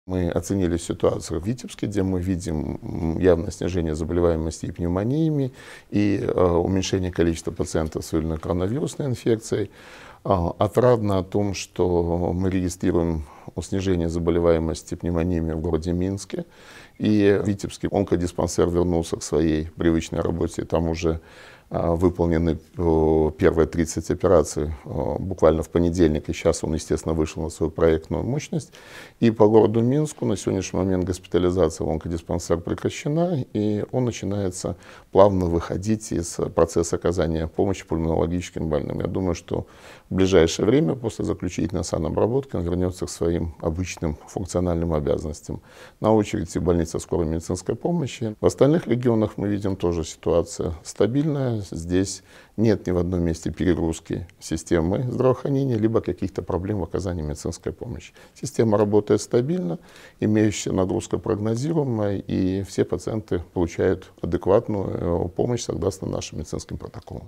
В Минске регистрируется снижение заболеваемости пневмониями - Караник
В Минске регистрируется снижение заболеваемости пневмониями. Об этом министр здравоохранения Владимир Караник заявил сегодня журналистам.